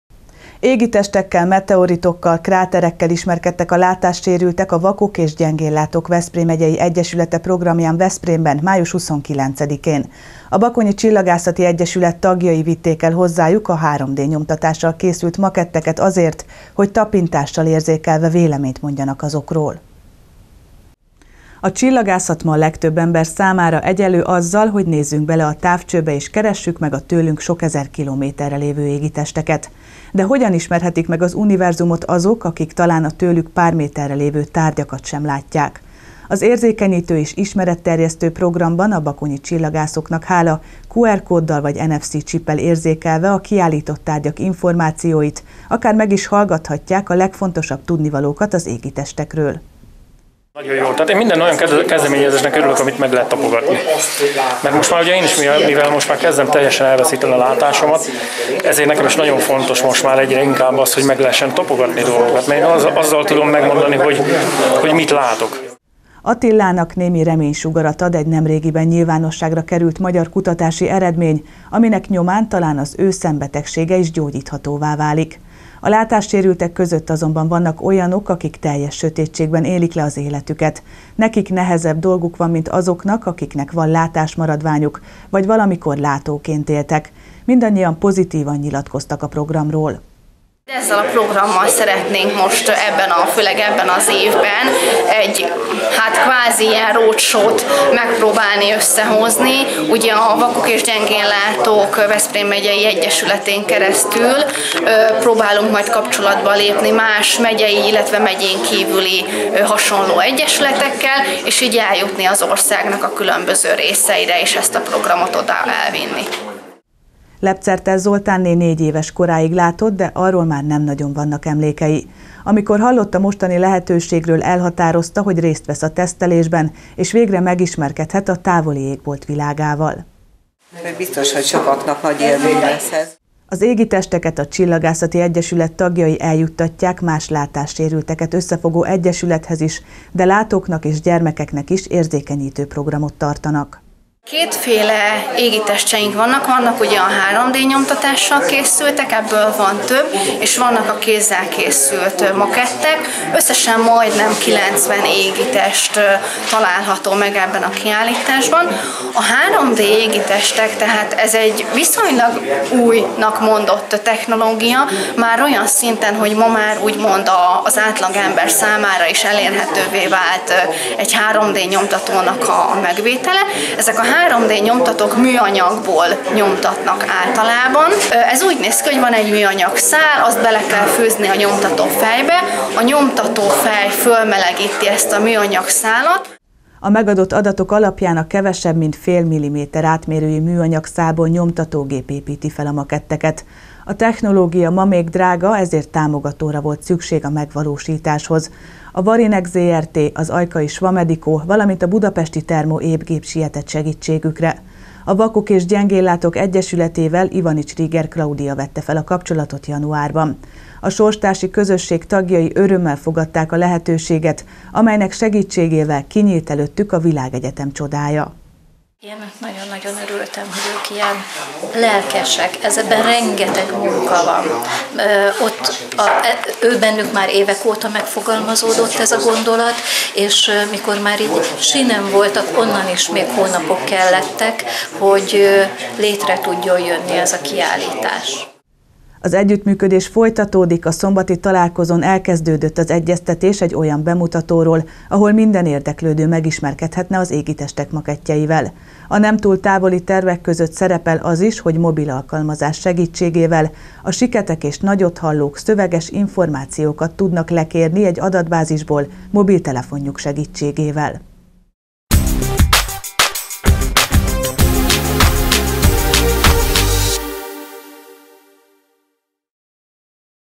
Látássérültekkel tesztelte az égitesteket a csillagászati egyesület
Égitestekkel, meteoritokkal, kráterekkel ismerkedtek a látássérültek a Vakok és Gyengénlátók Veszprém Megyei Egyesülete programján Veszprémben, május 29-én. A Bakonyi Csillagászati Egyesület tagjai vitték el hozzájuk a 3D nyomtatással készült maketteket azért, hogy tapintással érzékelve véleményt mondjanak azokról.